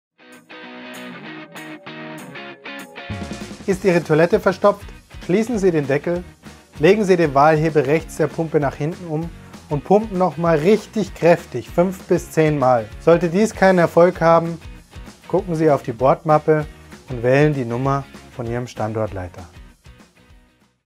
BunBo Hilfe: Die Toilette
Ihr seid mit einem unserer BunBos unterwegs und habt Fragen oder ein Problem? In unseren Hilfe-Videos zeigen wir euch einige Tricks und welche Lösungen ihr bei Problemen selbst ausprobieren könnt. Wir zeigen euch in diesem Video wie ihr eine verstopfte Toilette eventuell wieder frei bekommt.